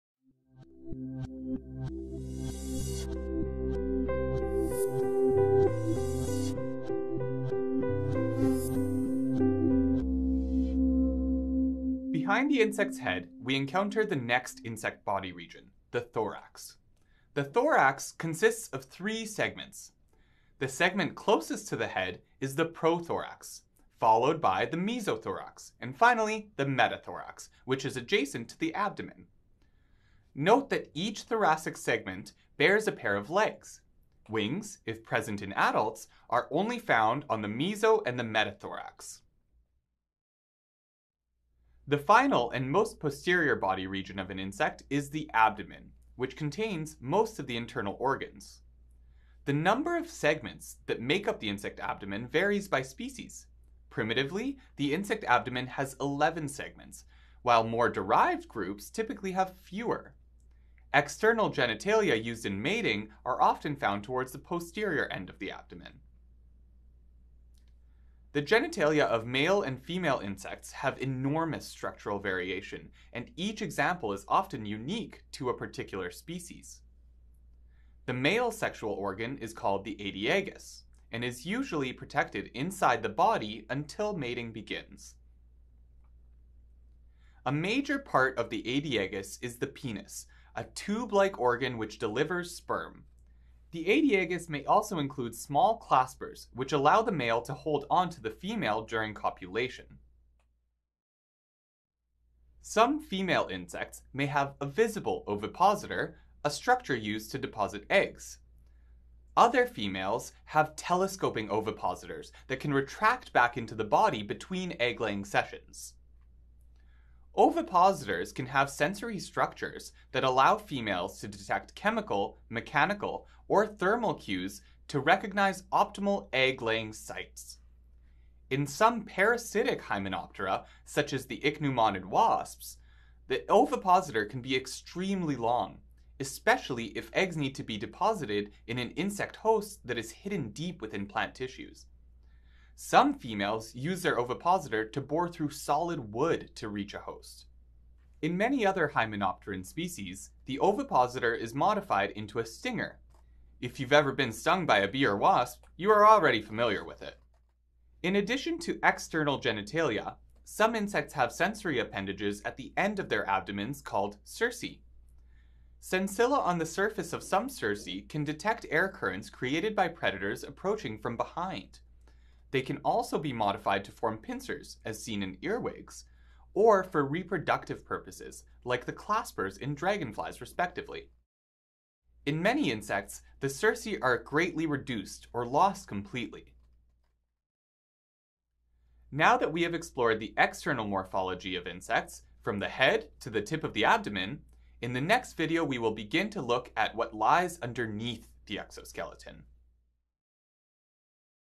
Insect thorax and abdomen | insects body parts | entomological hub | Module 02.03
entomological hub
this video contains  insect body region,  thorax, abdomen, prothorax, mesothorax, metathorax, External genitalia, aedeagus, Ovipositors, parasitic Hymenoptera such as the ichneumonid wasps, cerci. sensilla...

MODULE 02...